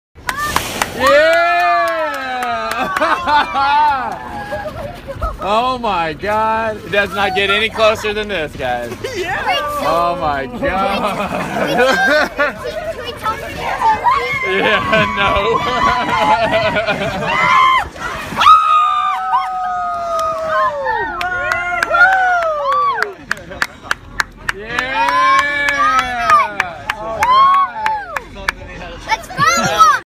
Whale watch on the Hawaii Nautical catamaran
We were out on a catamaran for a whale watching trip with Hawaii Nautical. As we were sitting still we were surprised by two whales off the back of the boat. Watch the entire clip to see the tails.